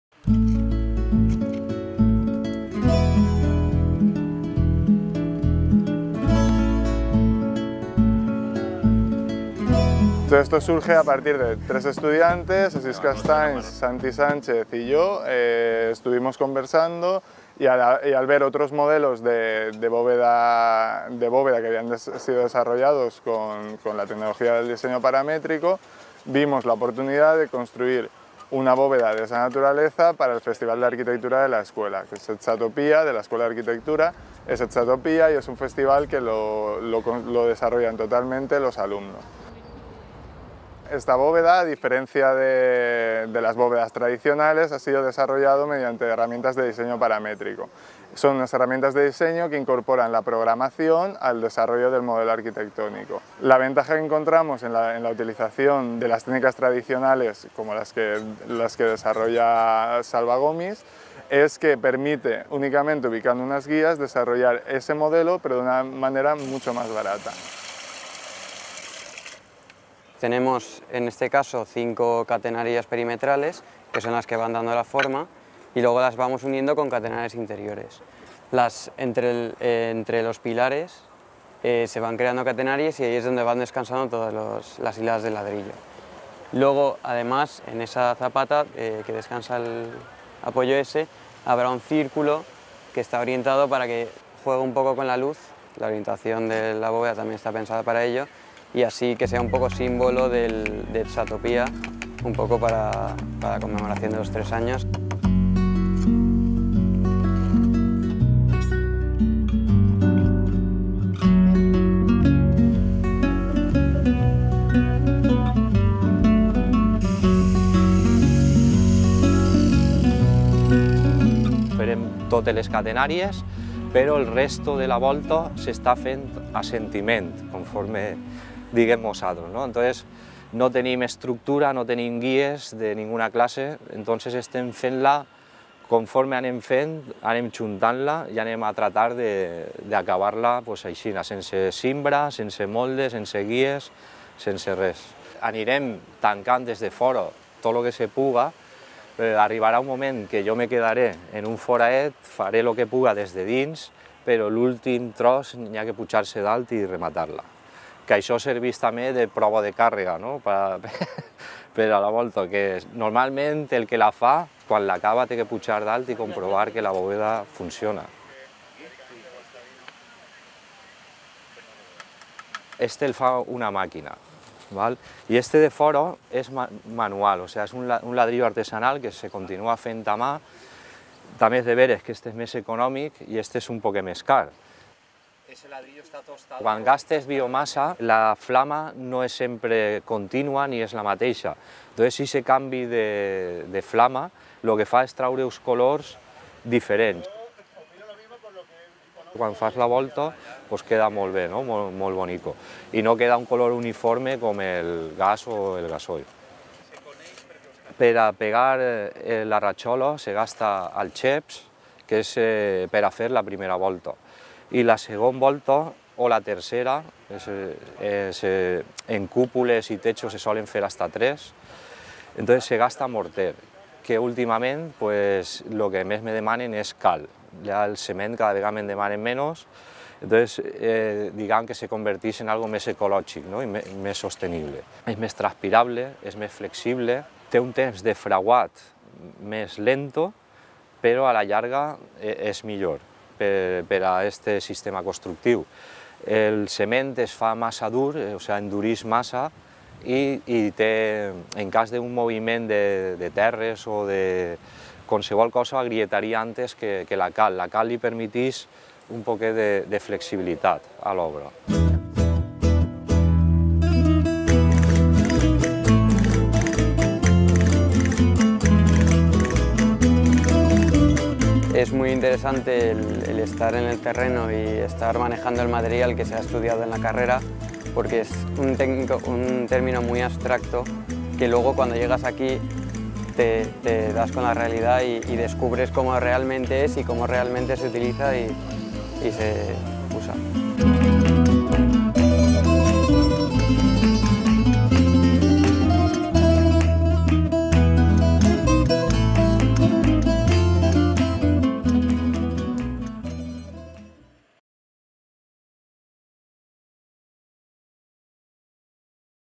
Reportaje: Bóvedas del pasado para el futuro / Voltes del passat per al futur
A voltes la solució per al futur és en el passat. Mirant enrere es poden trobar propostes arquitectòniques més barates i segures. Això és el que han fet a l’Escola Tècnica Superior d’Arquitectura de la Universitat Politècnica de València. Han recuperat les voltes de rajola de pla. Una solució arquitectònica que trobem a les termes romanes de Caracalla, del segle III, i que l’arquitecte valencià Rafael Guastavino exportà als Estats Units d’Amèrica al segle XIX. Ara pots veure’n una de prop al campus de Vera de la UPV.

A veces, la solución para el futuro está en el pasado. Mirando atrás se pueden encontrar propuestas arquitectónicas más baratas y seguras. Esto es el que han hecho en la Escuela Técnica Superior de Arquitectura de la Universitat Politècnica de València. Han recuperado las bóvedas tabicadas. Una solución arquitectónica que encontramos a las termas romanas de Caracalla, del siglo III, y que el arquitecto valenciano Rafael Guastavino exportó a los Estados Unidos de América, en el siglo XIX. Ahora puedes ver una, de cerca, en el campus de Vera de la UPV.